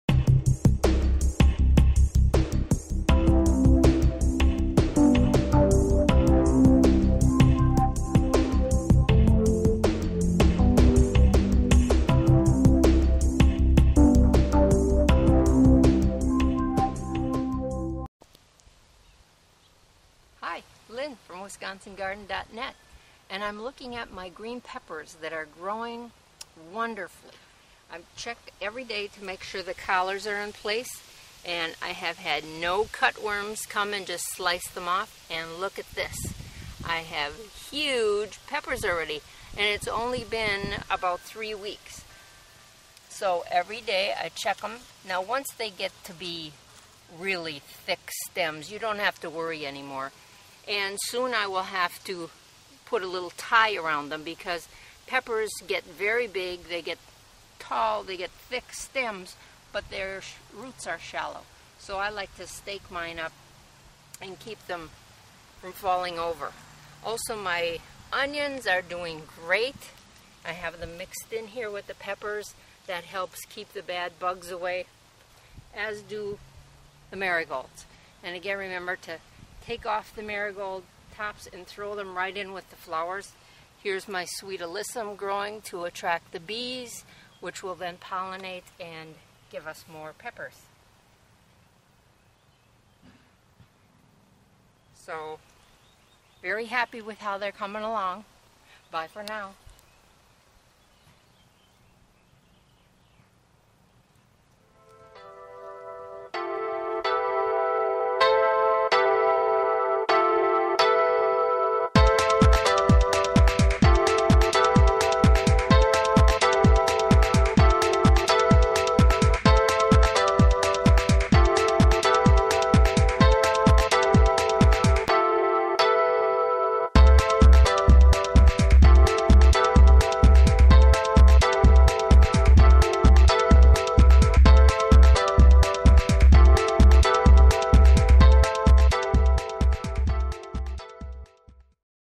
Baby Green Peppers - Wisconsin Garden Video Blog 141.avi
It has now been less than three weeks since we planted our pepper [lantws and here they come.  We love home-made salsa and can't wait to harvest our peppers, tomatoes, onions, and cilantro.